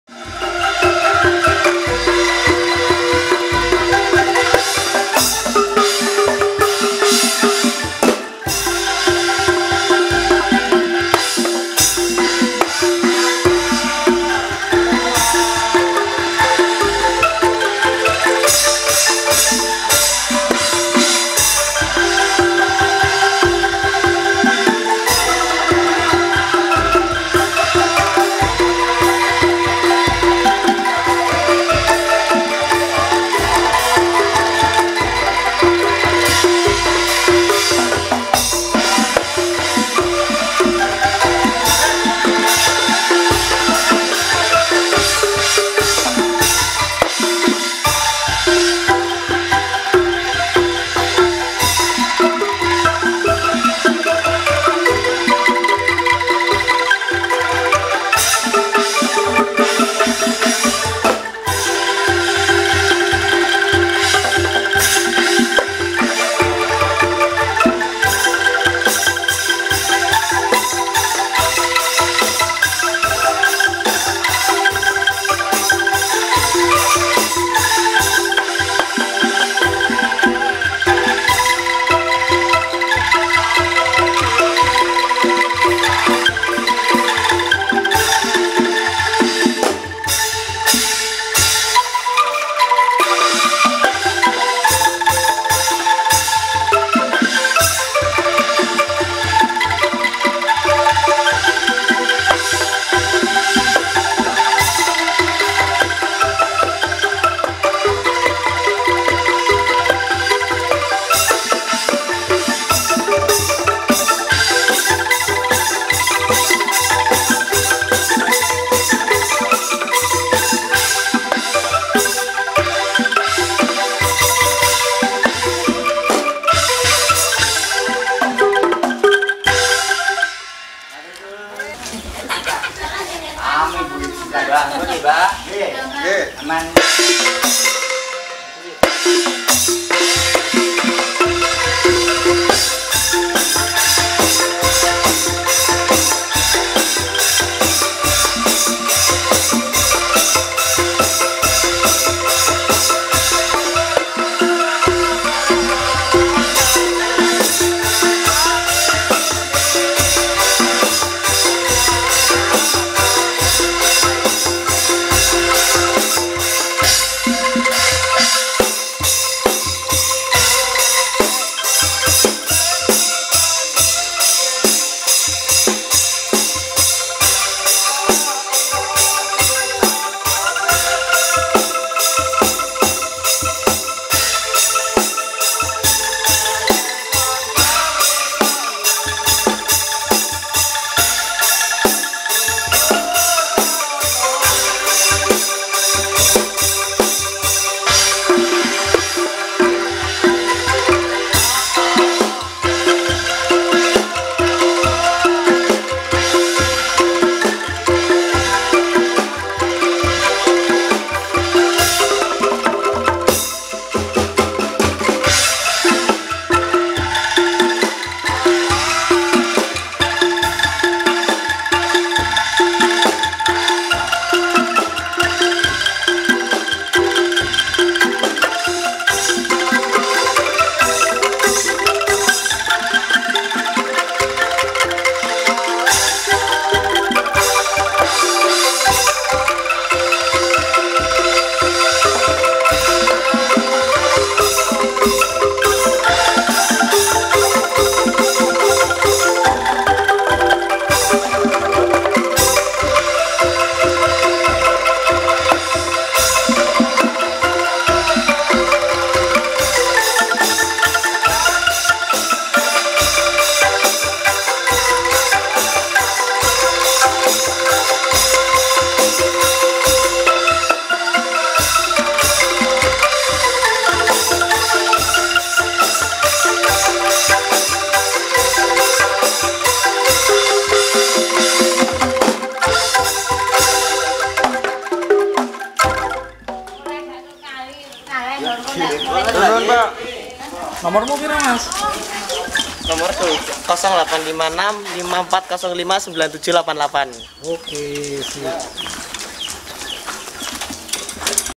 Nanggap Angklung Jalanan "New Nada Hore" Main Depan Rumah Warga Kasuran Wetan Margomulyo Seyegan
Buat yang ingin nanggap bisa hubungi nomor pemain angklung.

INFORMASI CP
Nama Grup : New Nada Hore
No HP : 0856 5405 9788
Akun Youtube : @newnadahore2022